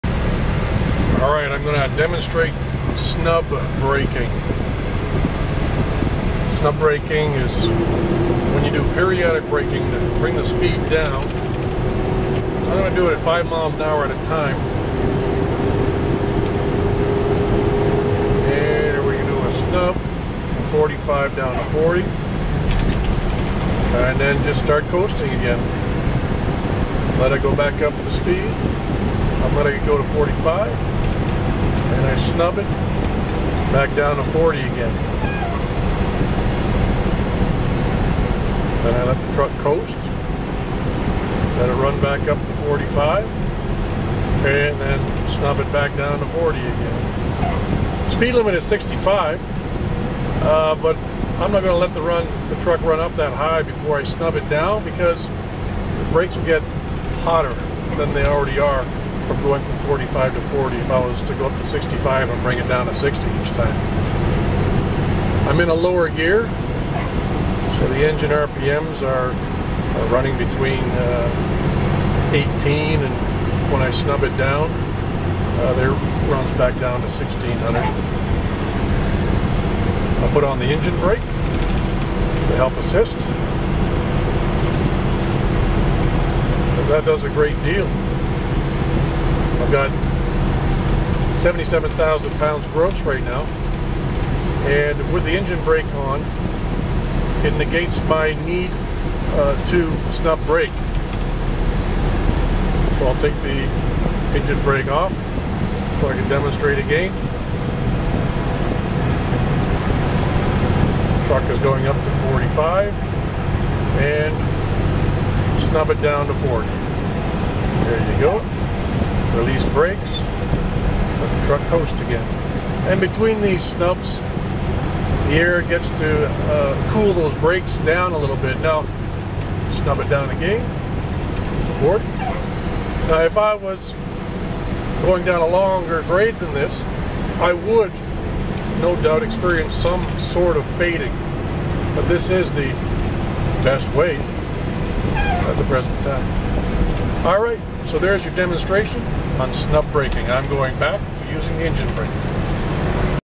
Mr. John Tutorial : Snub Braking
Demonstration of snub braking (and to lesser extent the effects of the jake brake) Apply firm pressure to slow the vehicle-but not so much as to lock the brakes up.